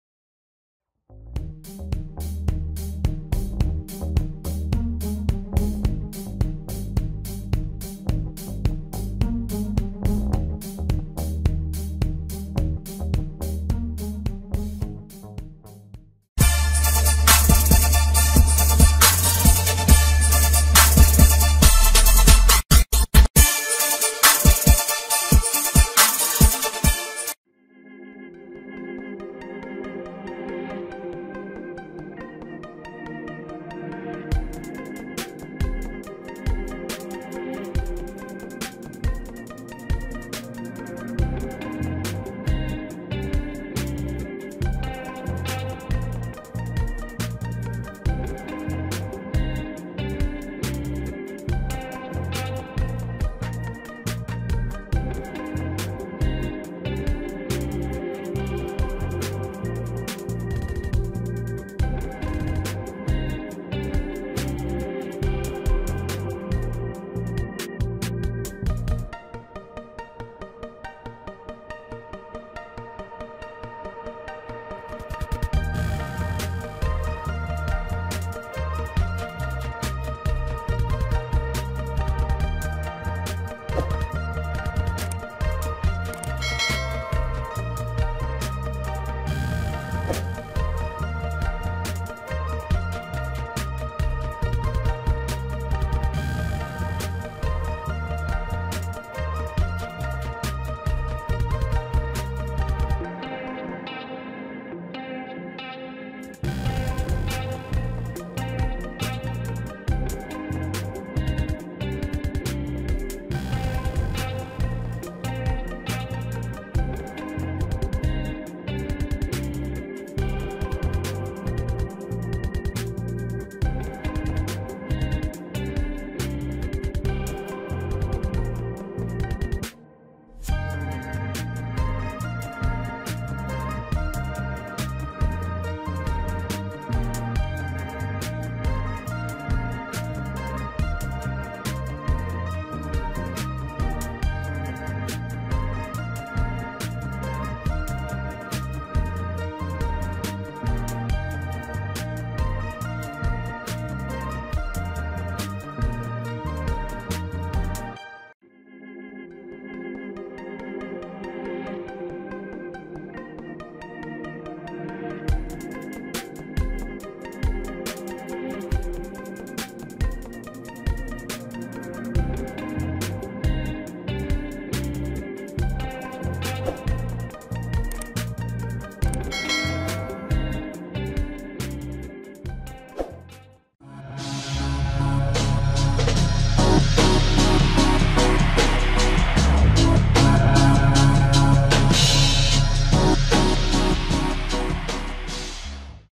how to block any application using firewall (sinhala)
Today lesson is abouth How to block a program using firewall.